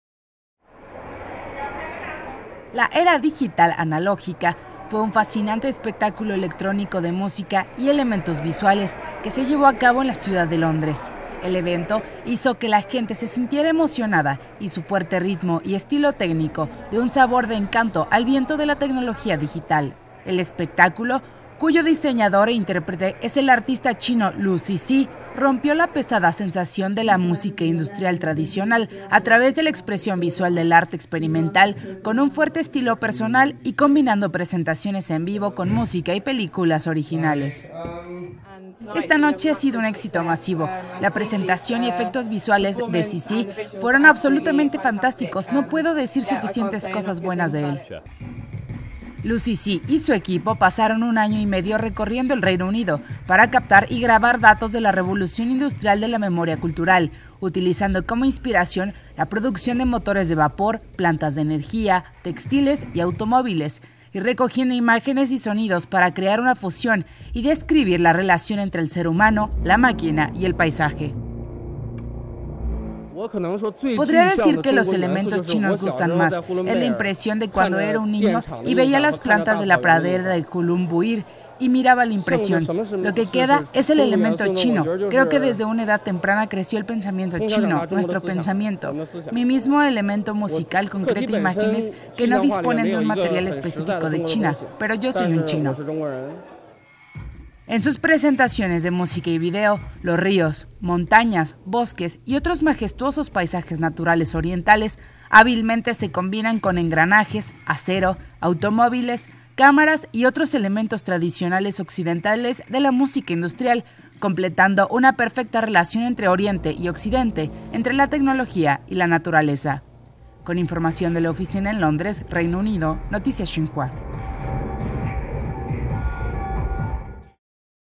Cuando la industria musical occidental se encuentra con la interpretación oriental
Lu Sisi utilizó como inspiración la producción de motores de vapor, plantas de energía, impresión, textiles y automóviles, robots industriales y otros elementos, y recogió imágenes y sonidos para crear una fusión y dejar fluir la música electrónica y las presentaciones de video, describiendo la relación entre el ser humano, la máquina y el paisaje